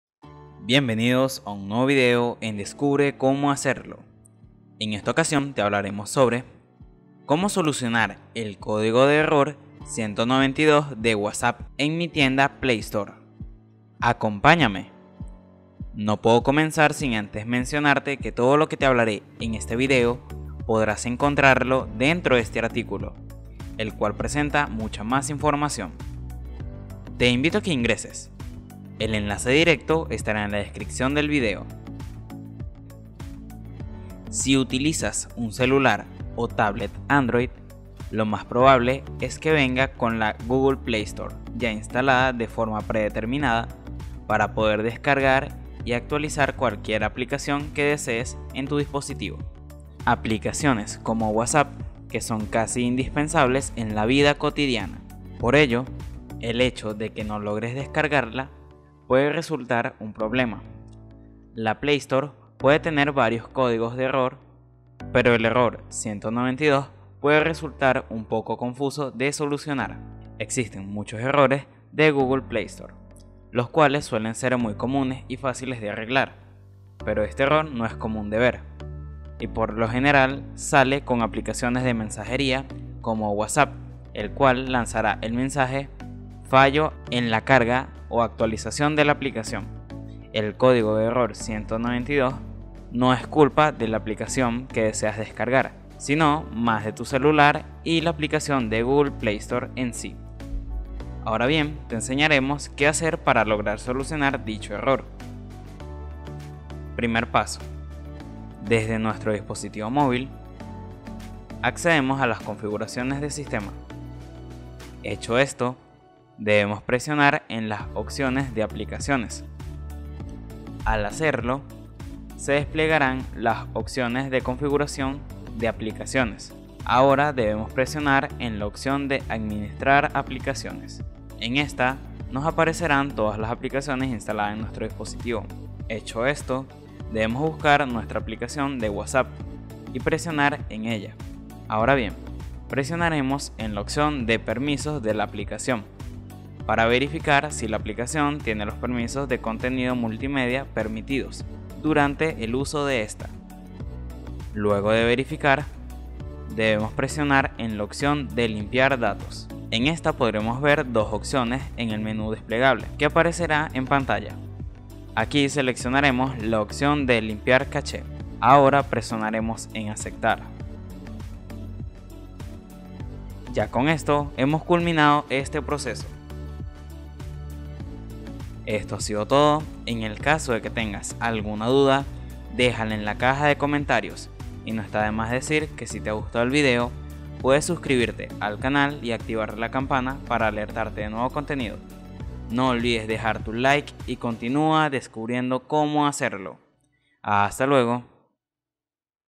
¿Cómo solucionar el código de error 192 de WhatsApp en mi tienda PLAY STORE?
En este video descubre Cómo solucionar el código de error 192 de WhatsApp en tu tienda PLAY STORE. En el Canal Descubre Cómo Hacerlo encontrarás el mejor contenido. ¡No te lo puedes perder!